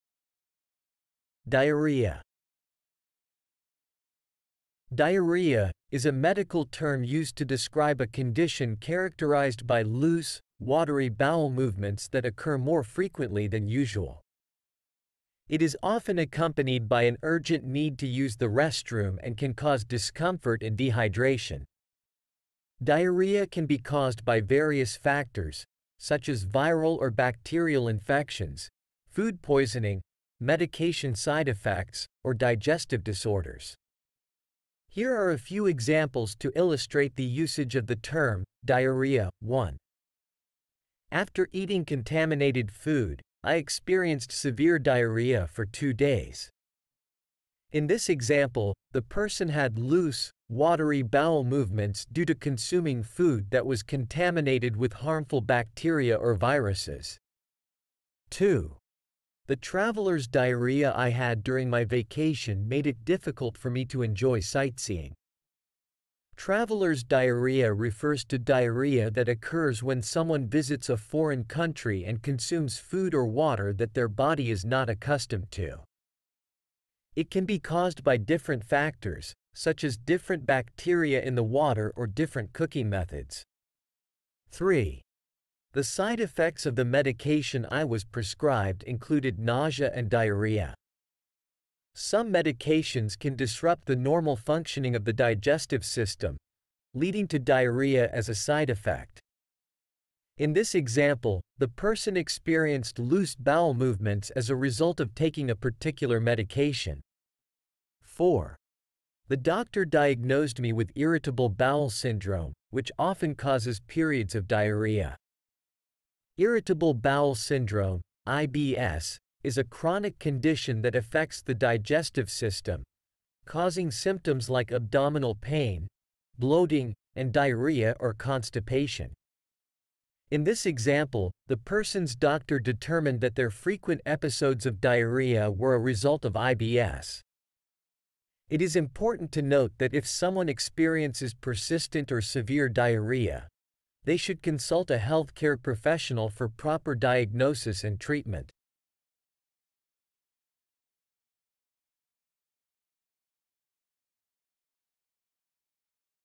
Diarrhea
"Diarrhea" is a medical term used to describe a condition characterized by loose, watery bowel movements that occur more frequently than usual. It is often accompanied by an urgent need to use the restroom and can cause discomfort and dehydration. Diarrhea can be caused by various factors, such as viral or bacterial infections, food poisoning, medication side effects, or digestive disorders.
Here are a few examples to illustrate the usage of the term "diarrhea":
1. "After eating contaminated food, I experienced severe diarrhea for two days."
   - In this example, the person had loose, watery bowel movements due to consuming food that was contaminated with harmful bacteria or viruses.
2. "The traveler's diarrhea I had during my vacation made it difficult for me to enjoy sightseeing."
   - Traveler's diarrhea refers to diarrhea that occurs when someone visits a foreign country and consumes food or water that their body is not accustomed to. It can be caused by different factors, such as different bacteria in the water or different cooking methods.
3. "The side effects of the medication I was prescribed included nausea and diarrhea."
   - Some medications can disrupt the normal functioning of the digestive system, leading to diarrhea as a side effect. In this example, the person experienced loose bowel movements as a result of taking a particular medication.
4. "The doctor diagnosed me with irritable bowel syndrome, which often causes periods of diarrhea."
   - Irritable bowel syndrome (IBS) is a chronic condition that affects the digestive system, causing symptoms like abdominal pain, bloating, and diarrhea or constipation. In this example, the person's doctor determined that their frequent episodes of diarrhea were a result of IBS.
It is important to note that if someone experiences persistent or severe diarrhea, they should consult a healthcare professional for proper diagnosis and treatment.